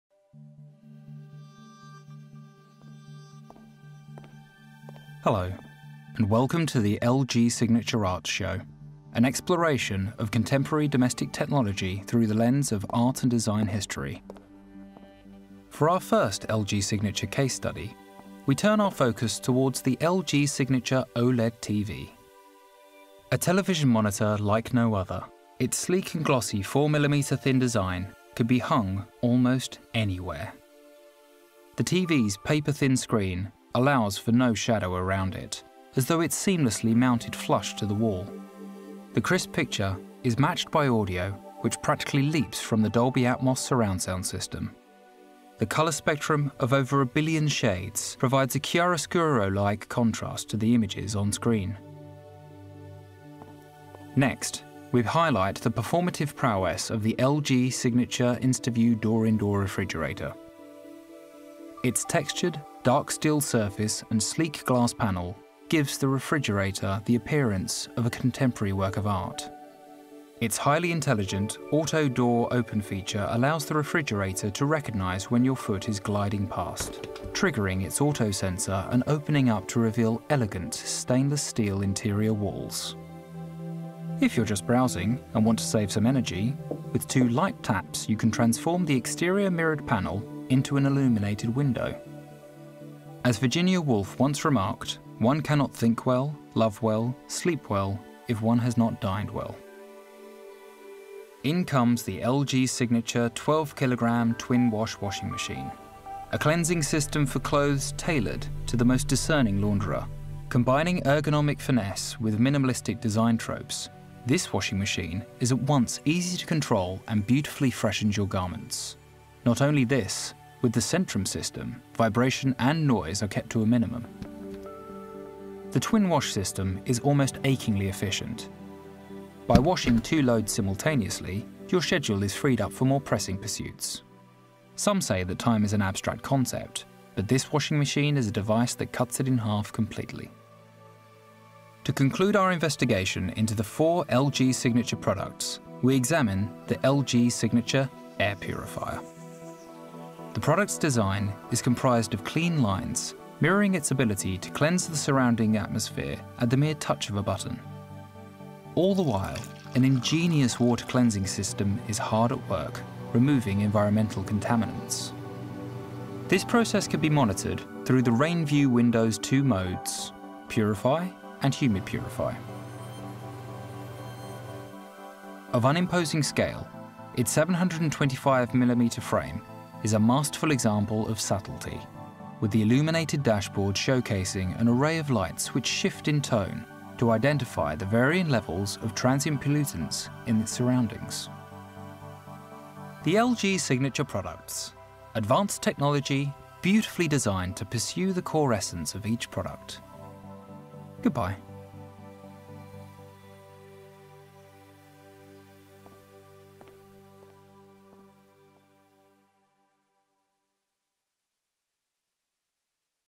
LG SIGNATURE x Wallpaper
“Uncompromised functional art.”

The perfect gift. Created specifically for you.
By someone who knows exactly who you are.

LG SIGNATURE is about more than just home appliances. It’s style and function that elevate your life.

Watch our collaboration with Wallpaper for a full overview of LG SIGNATURE’s star products